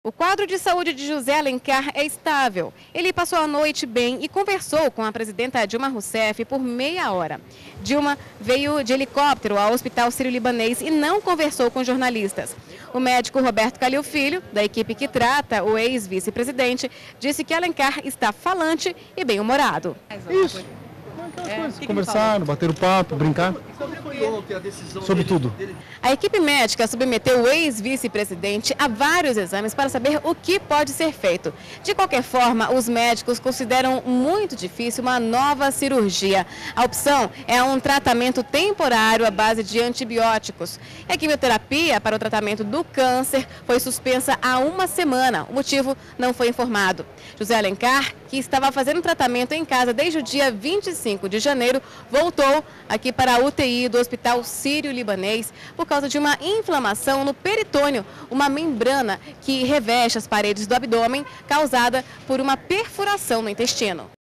José Alencar recebe visita da presidenta Dilma Rousseff
A presidenta Dilma Rousseff visitou o ex-vice-presidente José Alencar, no Hospital Sírio-Libanês, em São Paulo. Alencar voltou a ser internado por causa de uma perfuração no intestino.